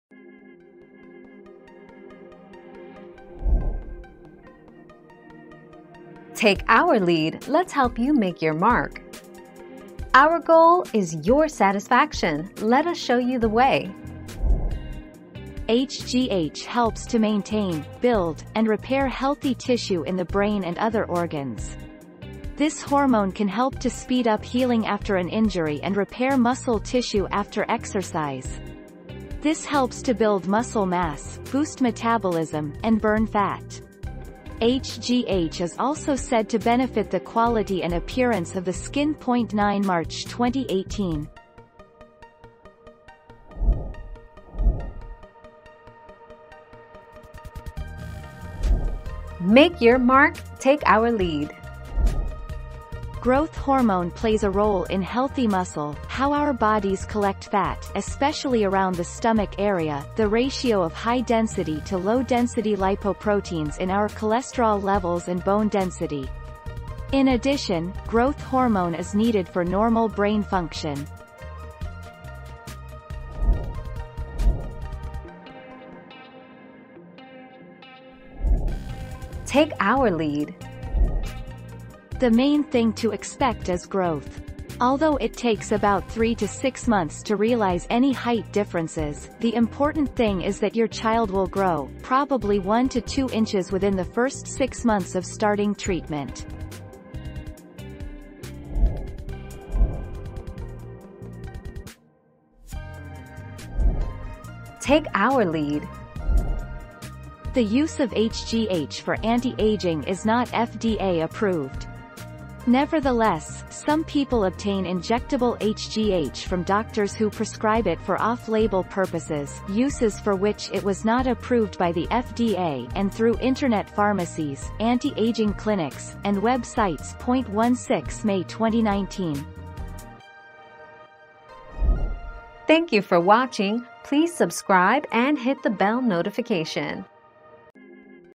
What are the benefits of HGH?
00:00 - What are the benefits of HGH?
00:45 - What does growth hormone do in adults?
01:15 - How long does HGH take to work?
01:40 - How can I get HGH legally?

Laura S. Harris (2021, February 9.) What are the benefits of HGH?
    AskAbout.video/articles/What-are-the-benefits-of-HGH-219418